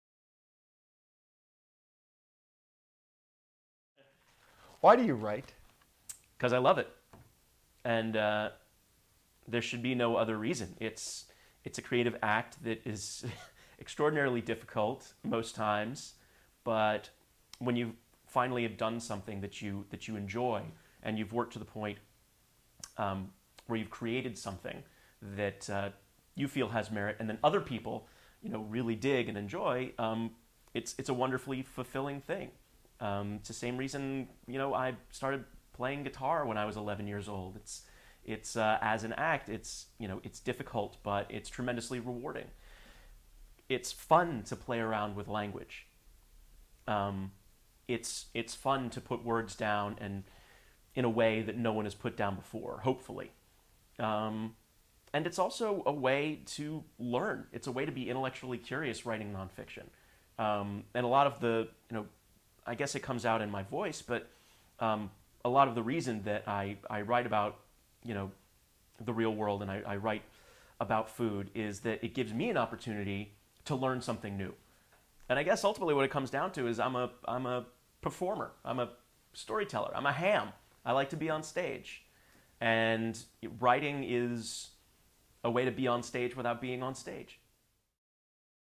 How Stage Performance Shaped Writing Career - Scott Gold
In Chapter 2 of 17, author Scott Gold speaks to how writing provides a performer a stage and the ability to harness passion for his or her curiosity, sharing, storytelling, and learning. Gold highlights how these ingredients connect his own love for non-fiction food writing, intellectual curiosity, and playing with language to his goal to entertain audiences.